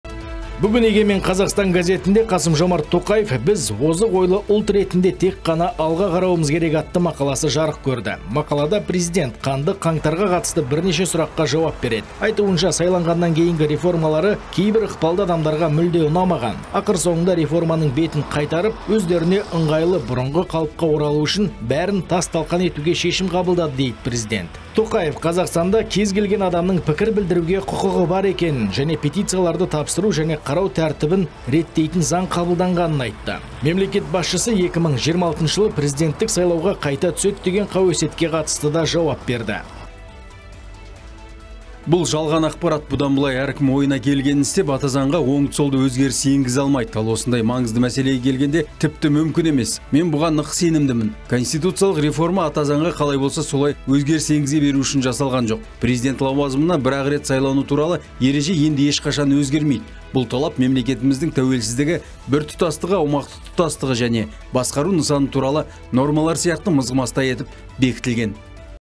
Қ.Тоқаев: Президент лауазымына бір-ақ рет сайлану туралы ереже енді ешқашан өзгермейді
Бүгін «Егемен Қазақстан» газетінде «Қасым-Жомарт Тоқаев: Біз озық ойлы ұлт ретінде тек қана алға қарауымыз керек» атты мақаласы жарық көрді. Мақалада Президент қанды қаңтарға қатысты бірнеше сұраққа жауап береді. Айтуынша, сайланғаннан кейінгі реформалары кейбір ықпалды адамдарға мүлде ұнамаған. «Ақыр соңында реформаның бетін қайтарып, өздеріне ыңғайлы бұрынғы қалыпқа оралу үшін бәрін тас-талқан етуге шешім қабылдады»,- деді Президент. Тоқаев Қазақстанда кез келген адамның пікір білдіруге құқығы бар екенін және петицияларды тапсыру және қарау тәртібін реттейтін заң қабылданғанын айтты. Мемлекет басшысы 2026 жылы президенттік сайлауға қайта түседі деген қауесетке қатысты да жауап берді.